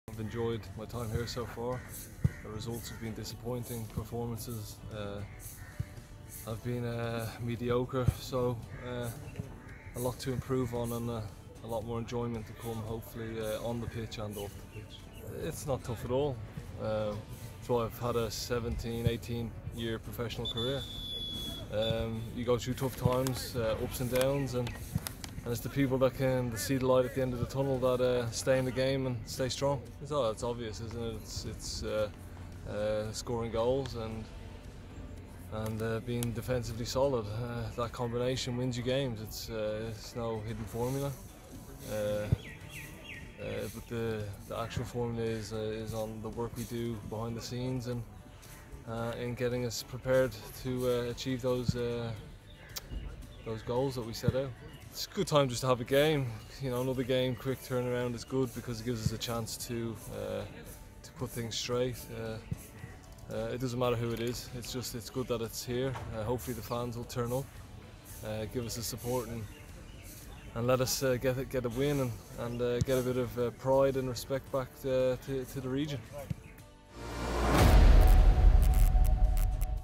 NorthEast United FC's Striker Andrew Keogh Previews the Match Against JFC
Andrew Keogh believes that the match against Jamshedpur FC has come at the right time for the club as this is the best time for them to play for their fans and regain their pride.